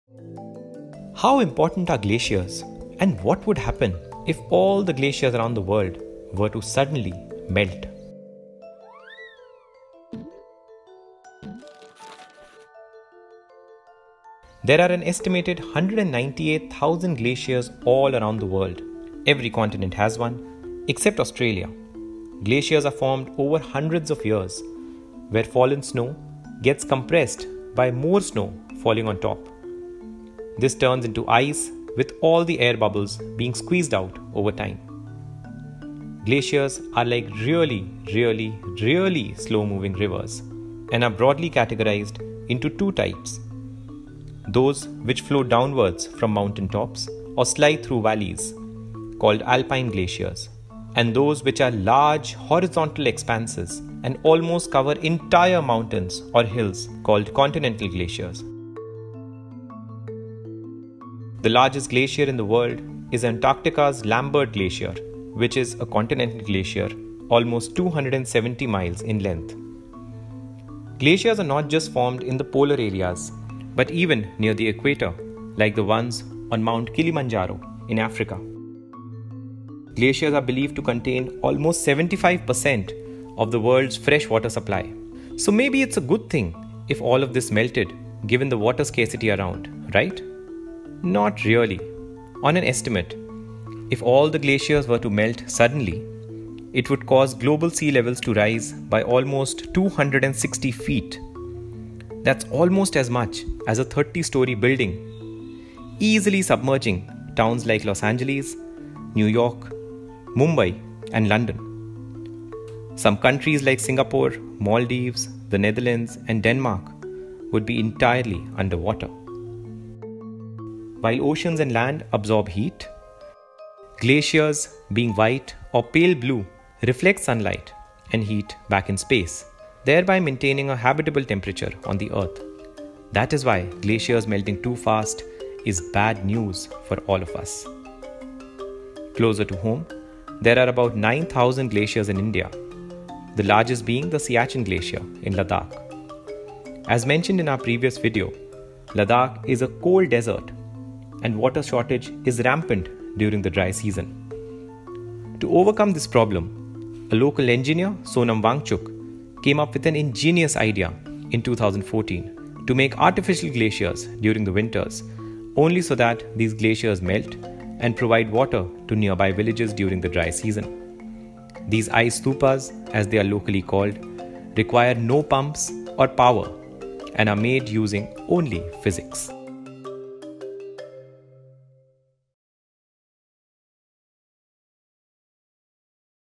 Glaciers - Earth's largest reserve of fresh water // Ep 11
Glaciers are found not just on Antarctica, but all over the earth, and they have a major role to play in our planet's response to its changing environment. So much so that man-made glaciers today that are helping serve their purpose.